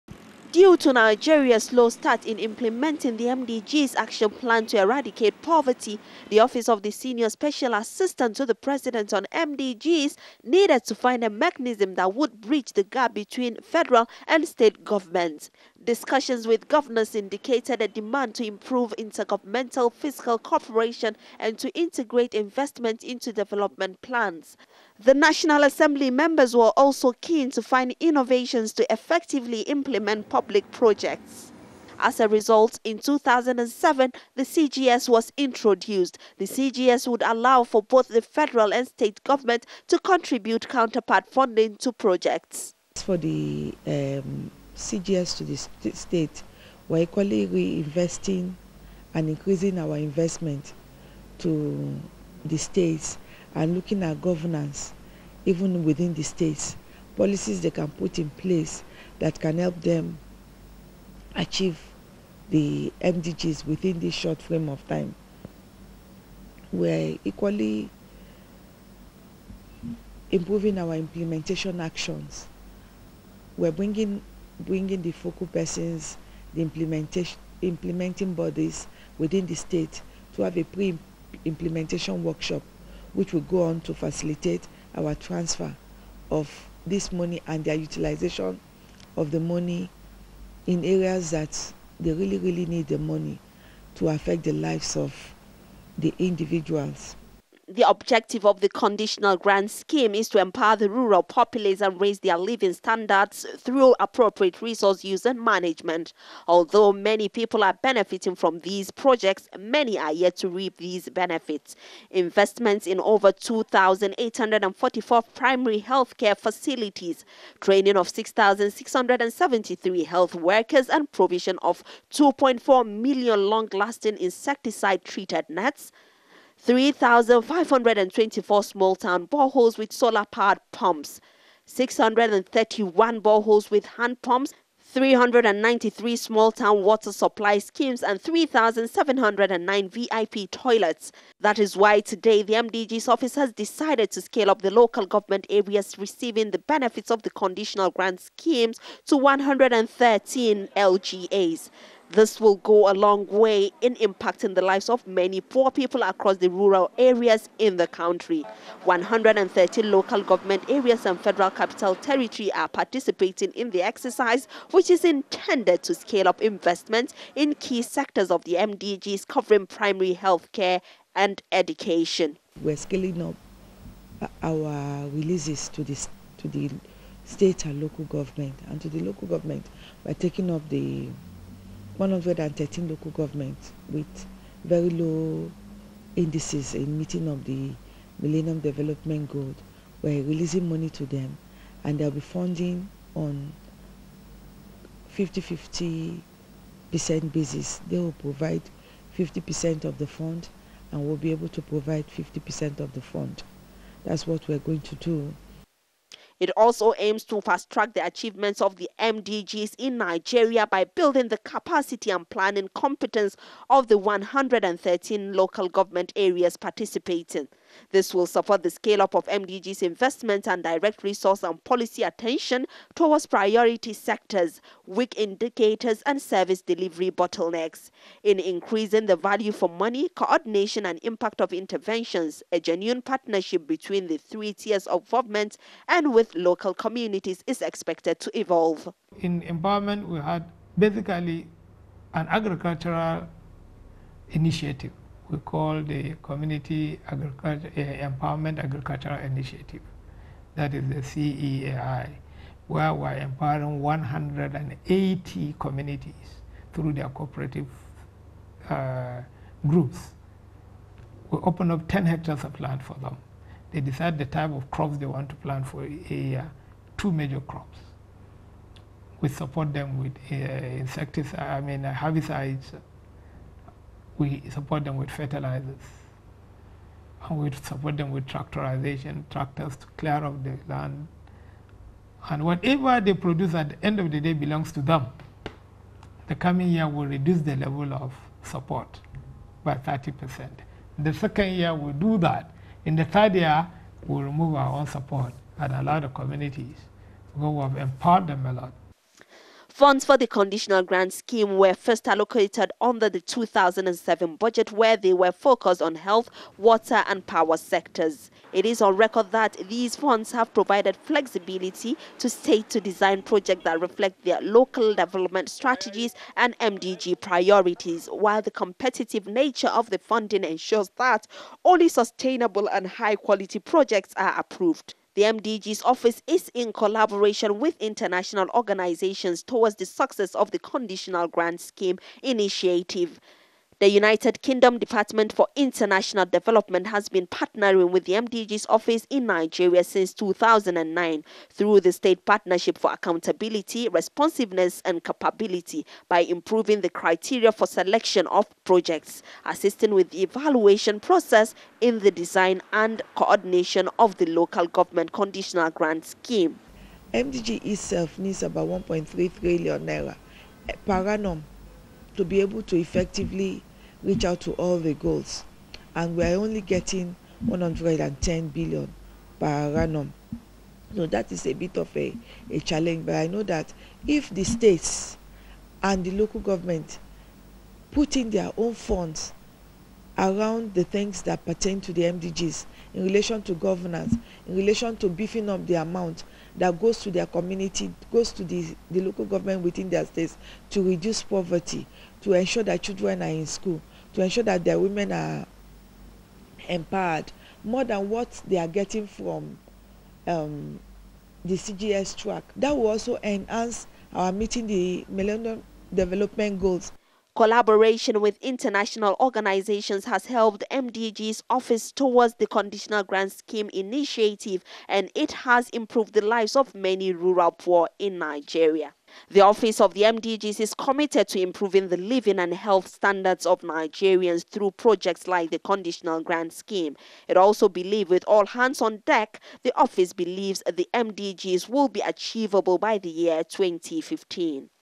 Grant Scheme LGA Scale up
In September 2005, the country successfully negotiated 18billion dollars debt relief package from the Paris Club of creditors, giving  rise to annual debt savings of about 1billion dollars. The Federal Government decided to channel these funds into pro-poor projects that would catalyse achievements of the MDGs.